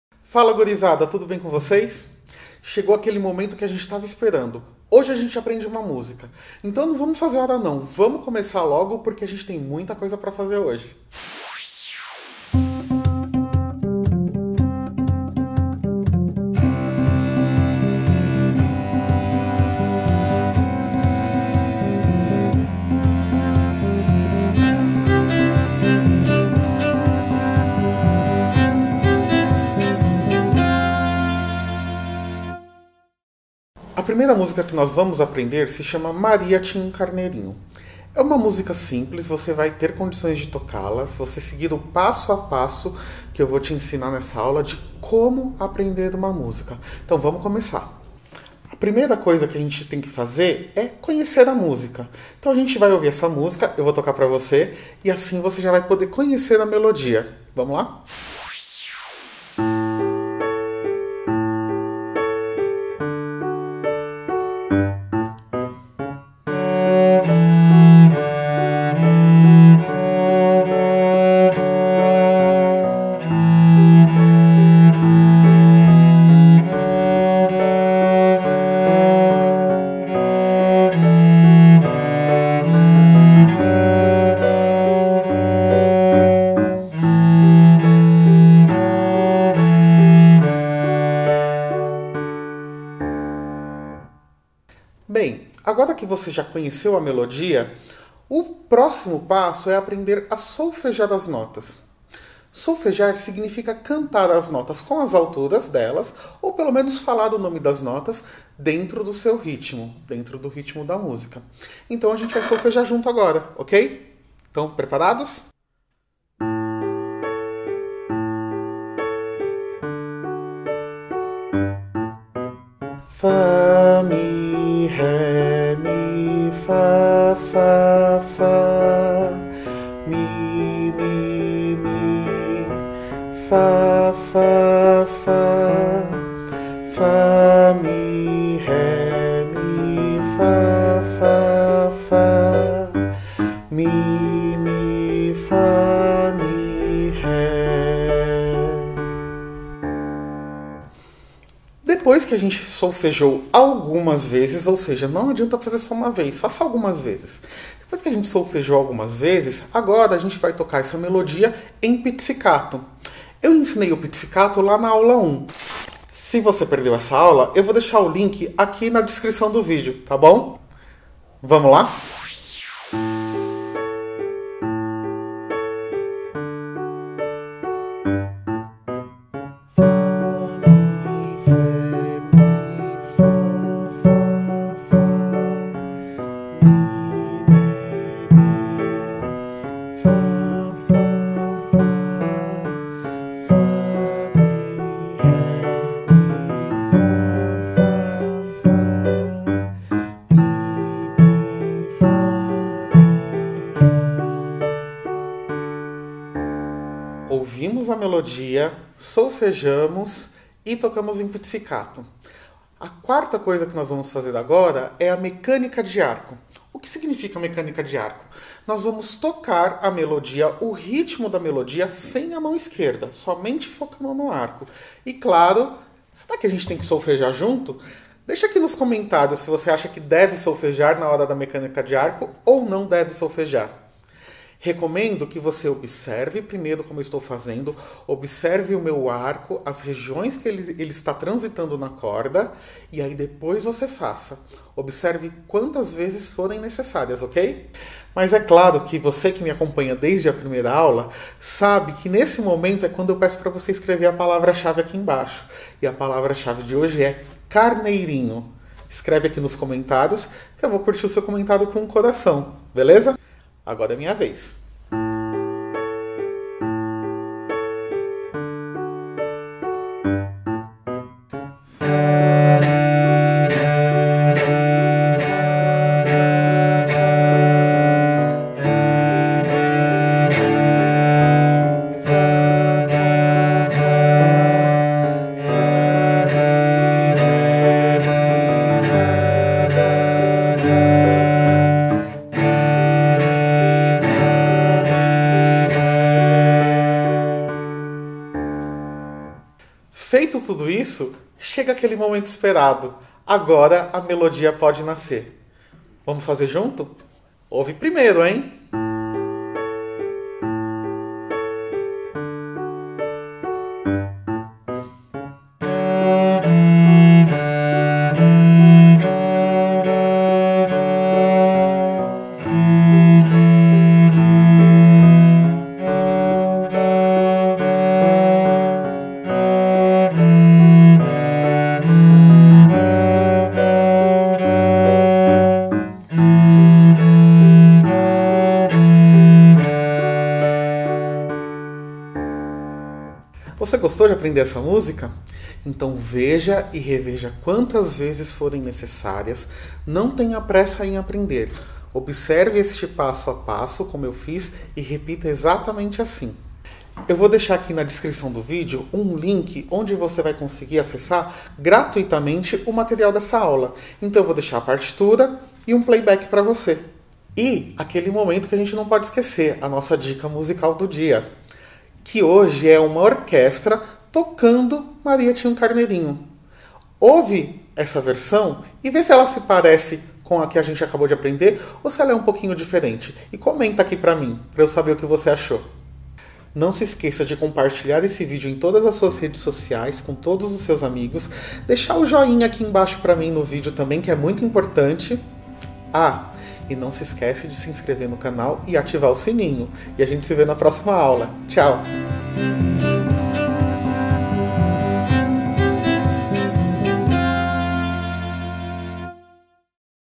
Violoncelo em 8 Passos | Aprendendo melodias fáceis
Alô Gurizada! 🔈

Chegou o momento tão esperado! Hoje a gente você aprende a tocar música no violoncelo! Com dicas simples e fáceis, você acompanha o passo a passo para tocar no violoncelo! Acompanhe a série: "Violoncelo em 8 Passos" com o professor Valdir Maia. Em 8 episódios, você irá aprender dicas importantes sobre o instrumento, sua história e como fazer o seu primeiro som!

Curtiu a aula?